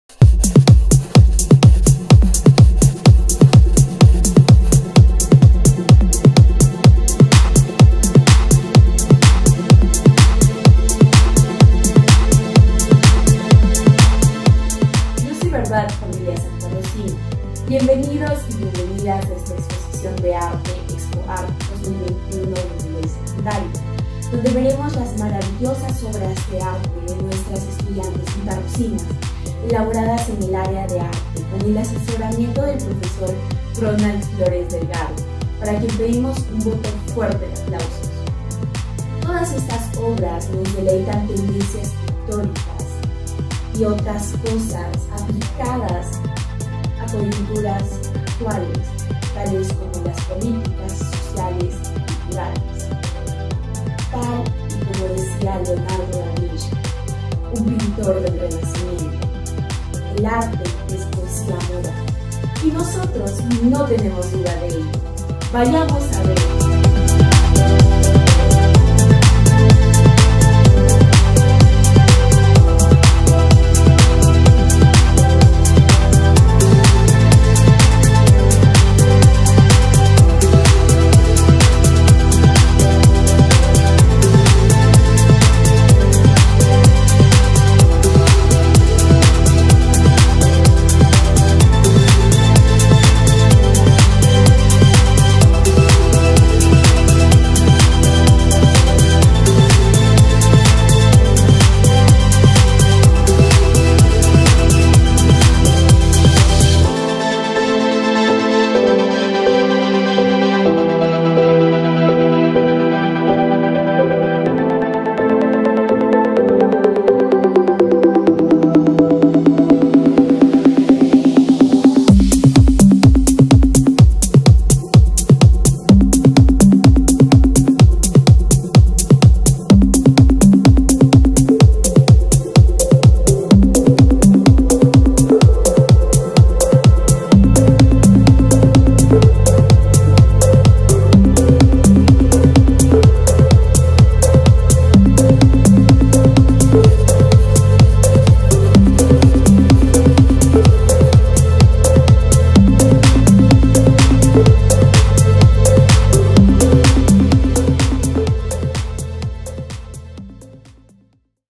𝗘𝘅𝗽𝗼 𝗮𝗿𝘁𝗲 𝗩𝗶𝗿𝘁𝘂𝗮𝗹 𝟮𝟬𝟮𝟭– 𝟯𝗿𝗼 𝗱𝗲 𝗦𝗘𝗖𝗨𝗡𝗗𝗔𝗥𝗜𝗔🎨👩‍🎨
𝗘𝘅𝗽𝗼𝗔𝗿𝘁𝗲𝗩𝗶𝗿𝘁𝘂𝗮𝗹𝟮𝟬𝟮𝟭 🎨👩‍🎨 compartimos los trabajos artísticos de nuestras estudiantes del 3er grado de secundaria. 👇🎥📸